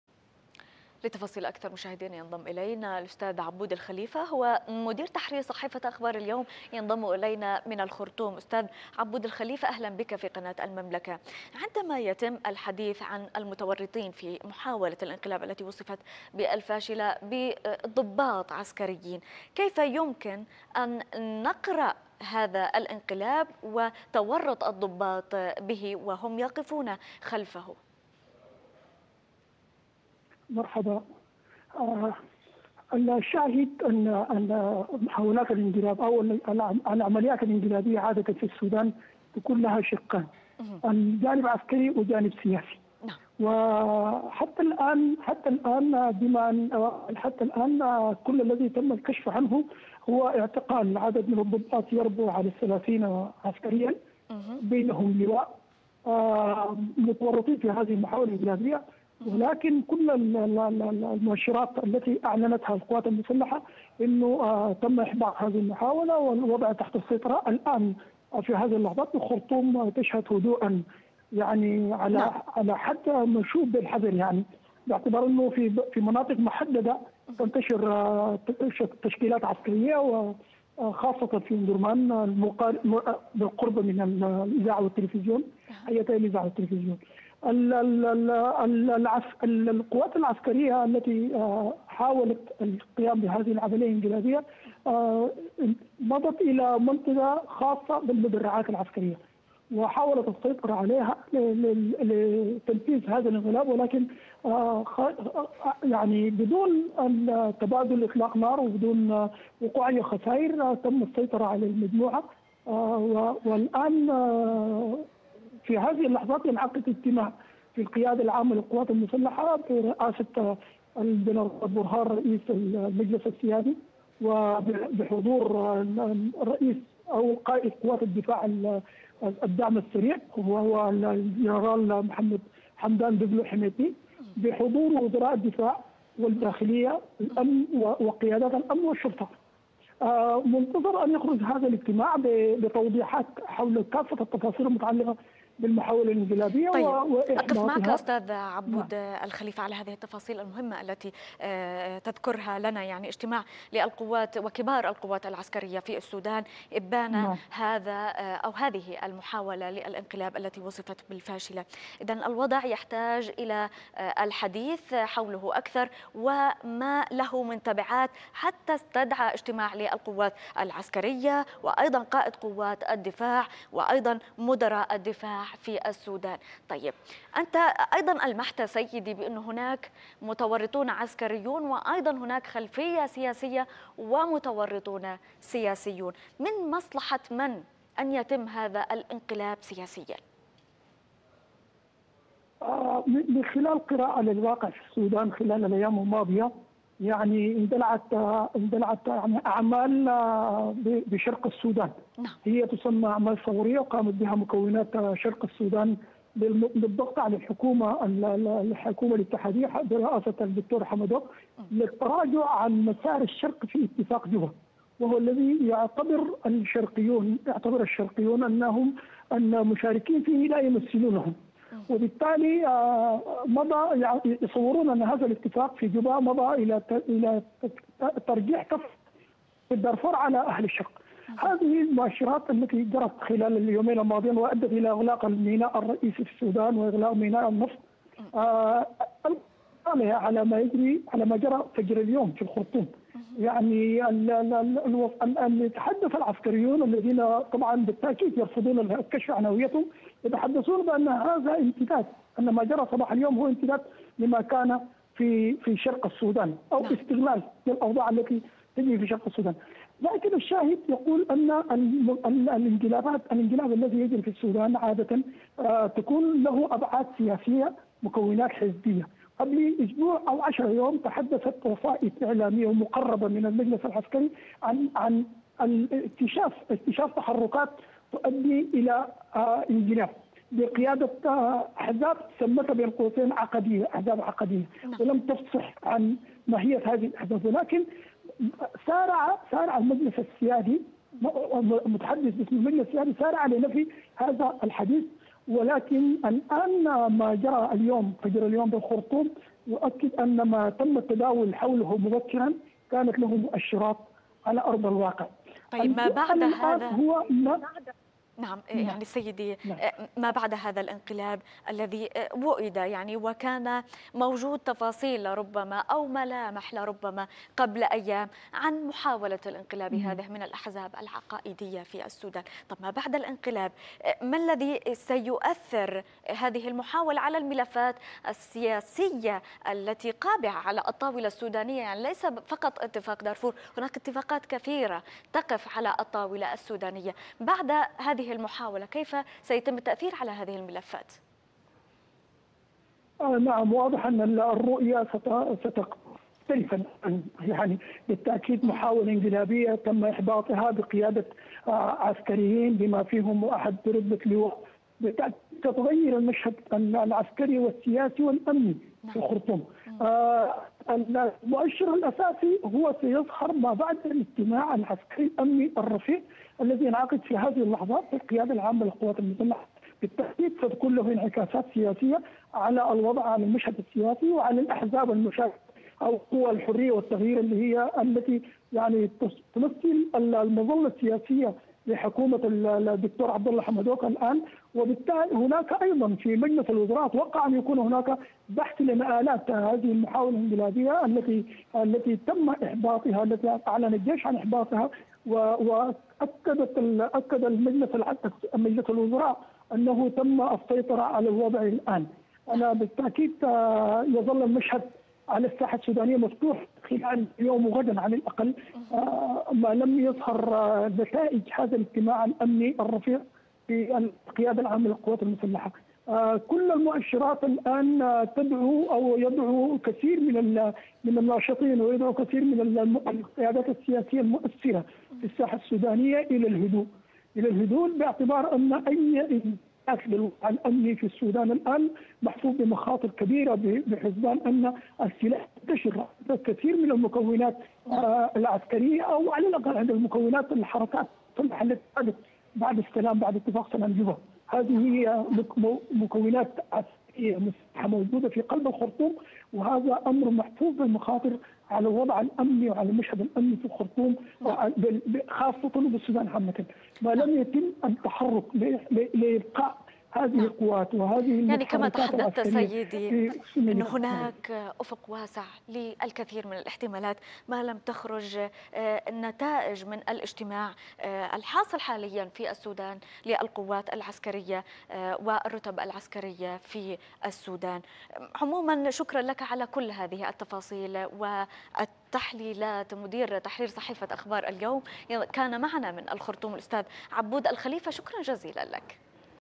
الجيش السوداني يعلن إحباطه محاولة انقلابية
/عبود الخليفة، مدير تحرير صحيفة أخبار اليوم